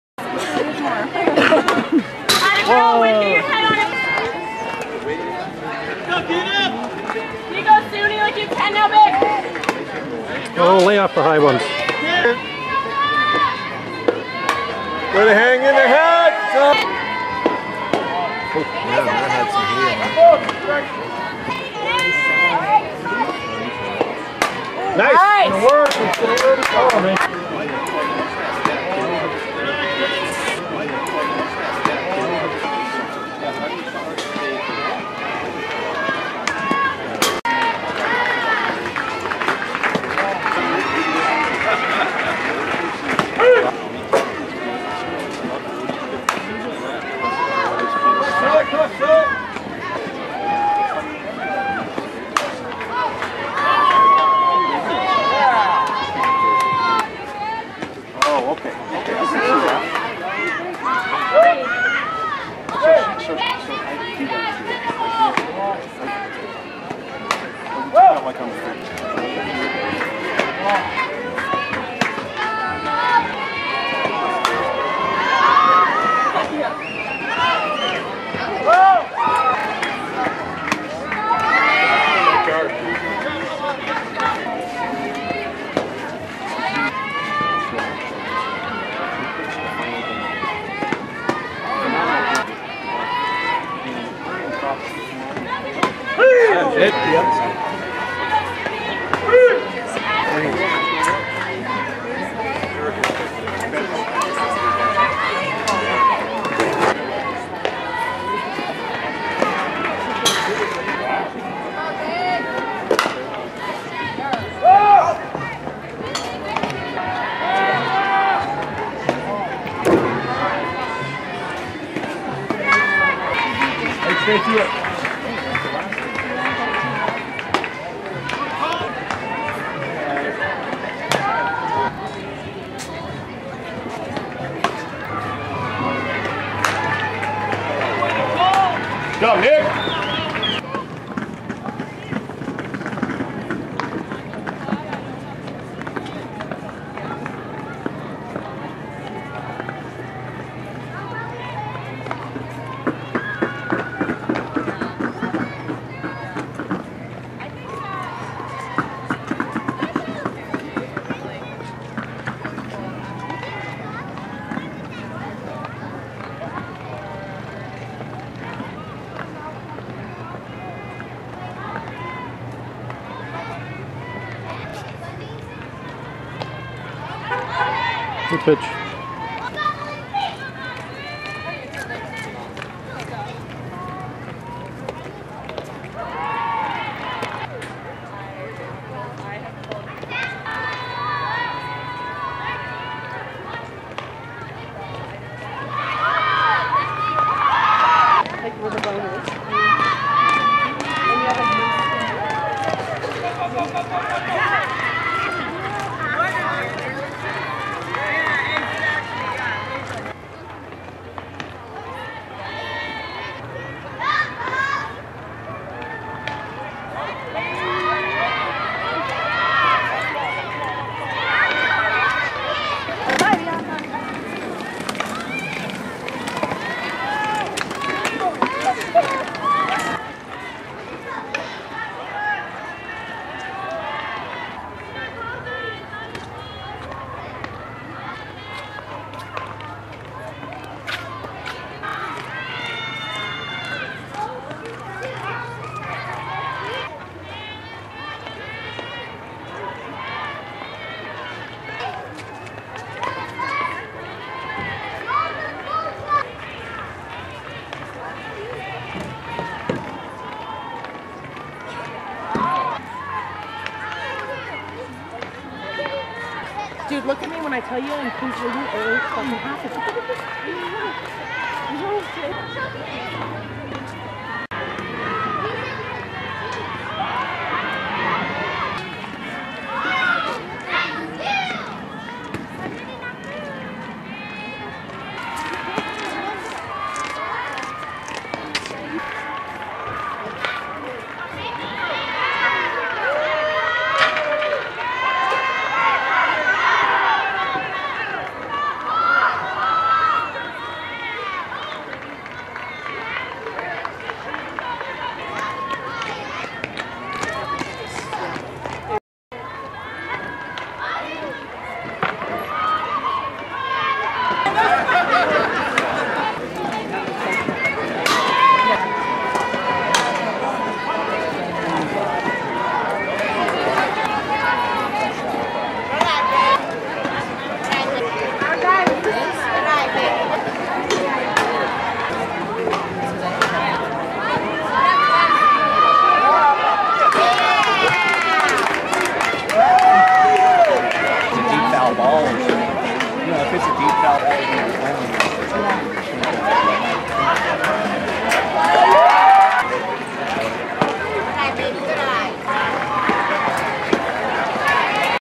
Vegas1_Suni
Suni pitching for Yale at the Vegas Showcase Oct 23 2014